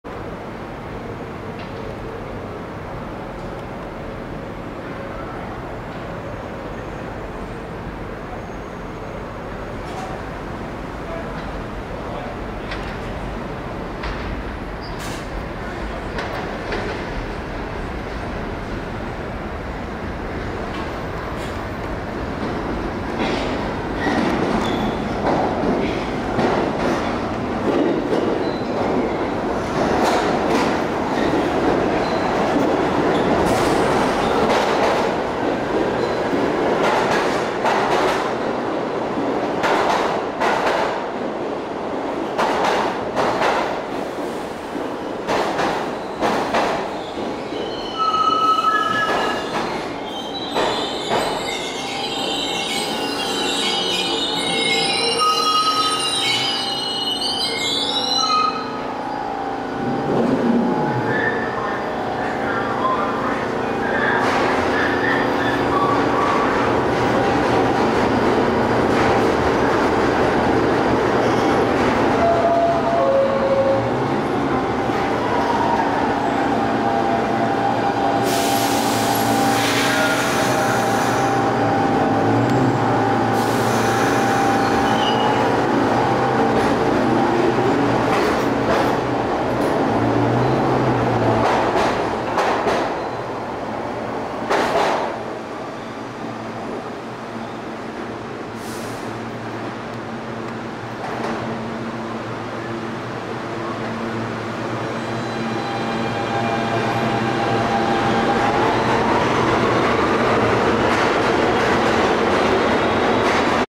MTA NYC Subway R train at Prince St
Whitehall St-bound MTA NYC Subway R train of Pullman-Standard R46 cars arriving at & leaving Prince St.
The R train branch to Whitehall St has since been recombined with the R train branch to Court St following the reopening of the Montague St Tunnel.
뉴욕 MTA 시티 지하철 R46호대 R선 화이트 홀 St행 열차
프린스 St역을 도착와 출발합니다.
This video was taken on July 28, 2014.